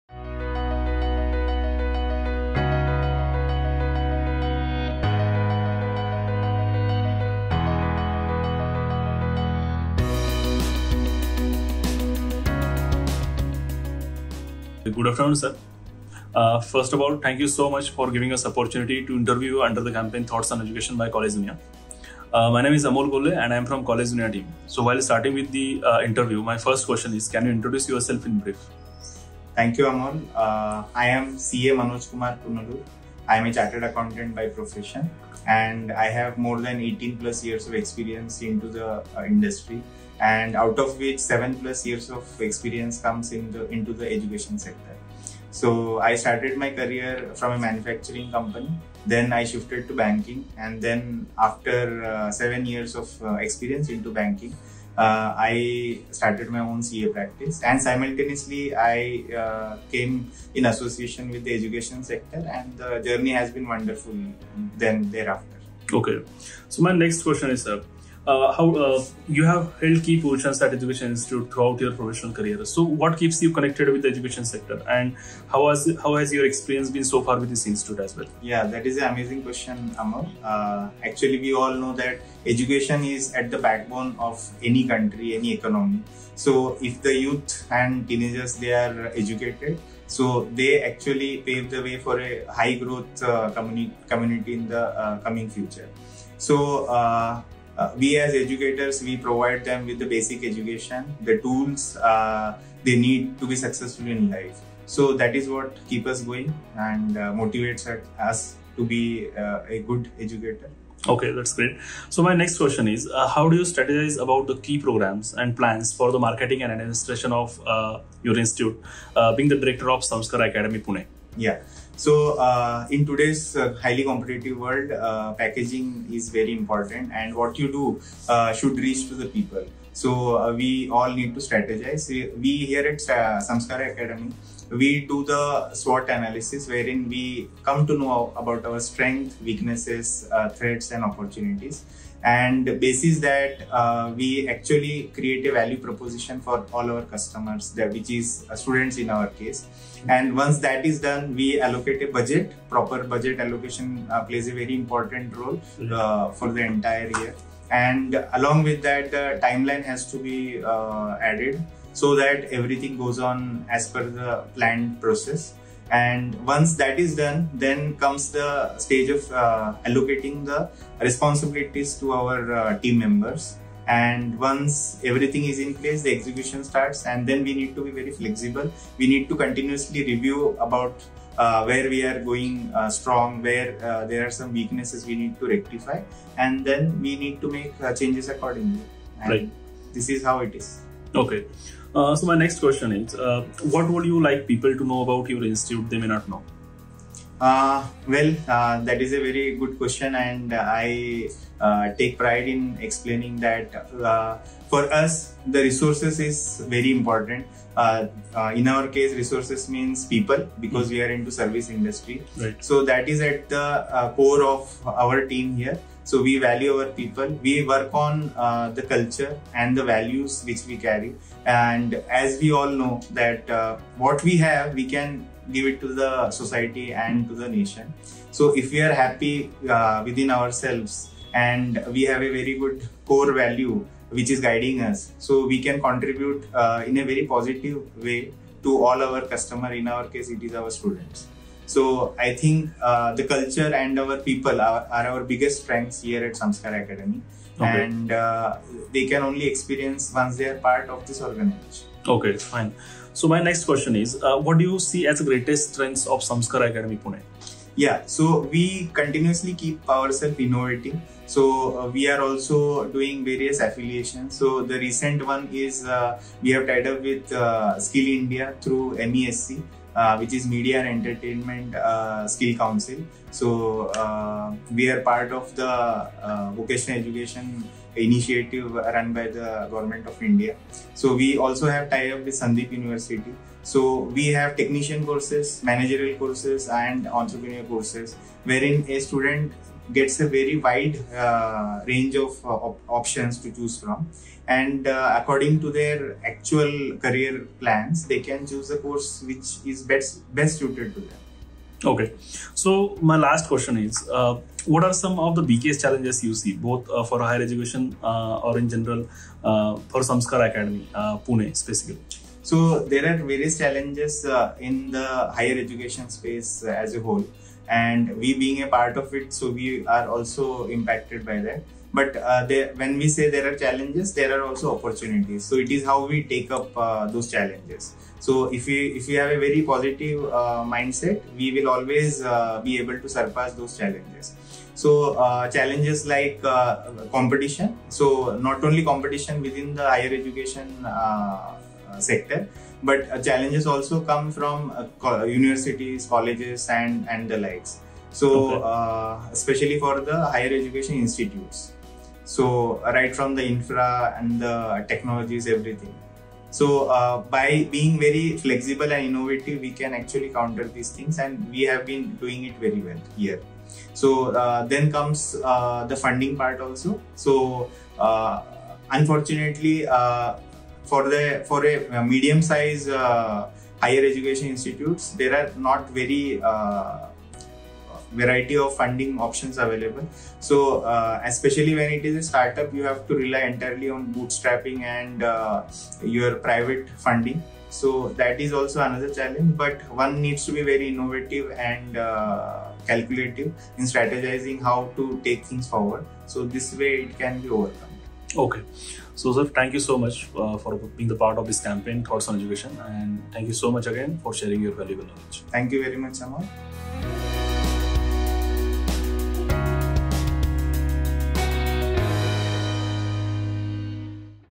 CA Manoj Kumar Punalur | Director | Samskara Acadmy, Pune
Next in our series of Thoughtful Leaders, we have, CA Manoj Kumar Punalur, Director, Samskara Acadmy, Pune